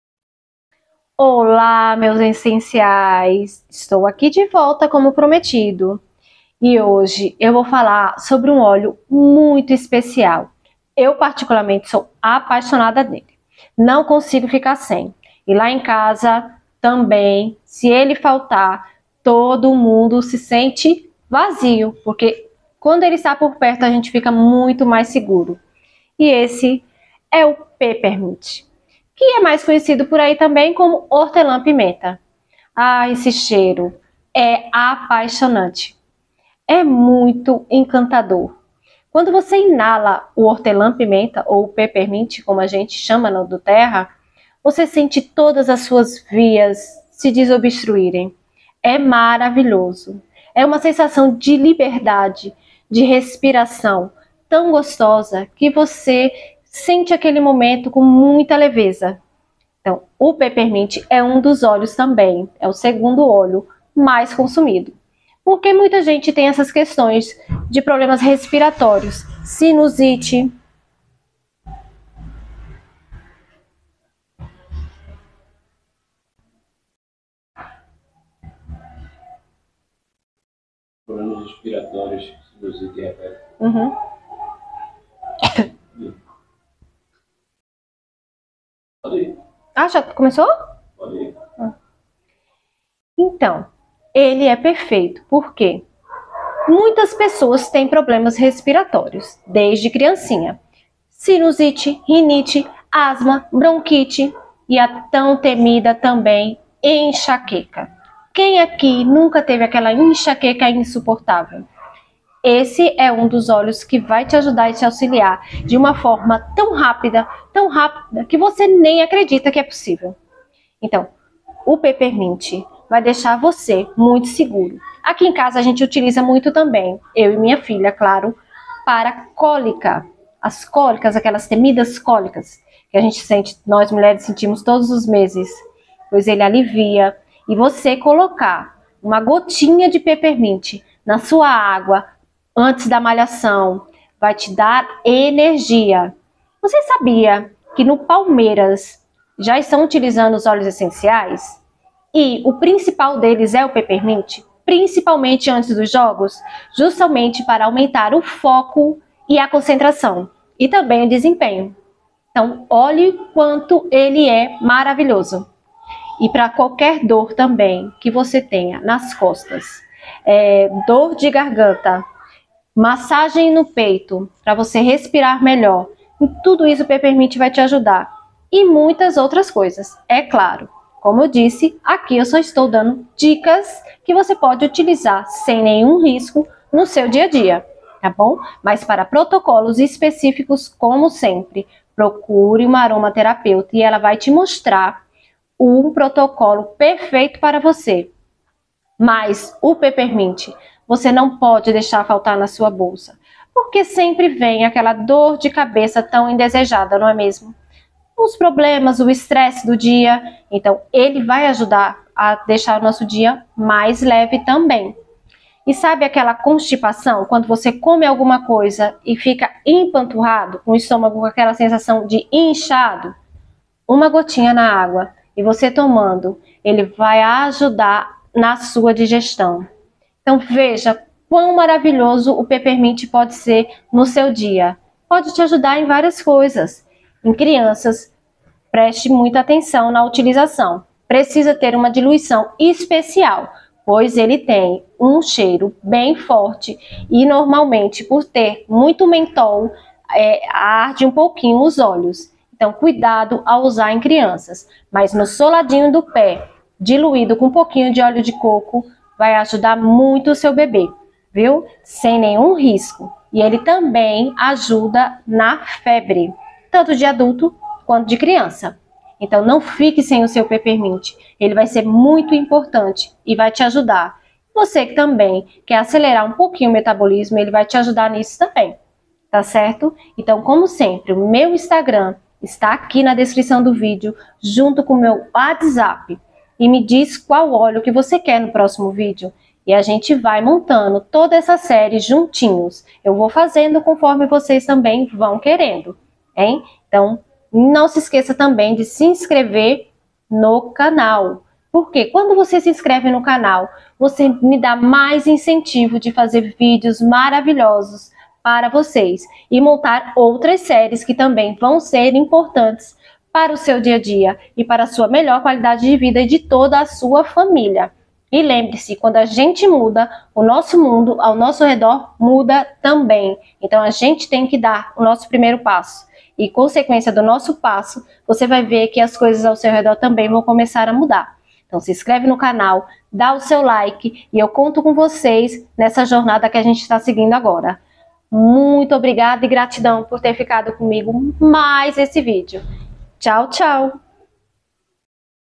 Óleo Essencial Peppermint (Hortelã Pimenta)
Hoje vamos aprender sobre como o peppermint pode impactar a sua vida positivamente, e o melhor de forma segura e natural. Deixa falar um segredo, esse é um dos meus preferidos.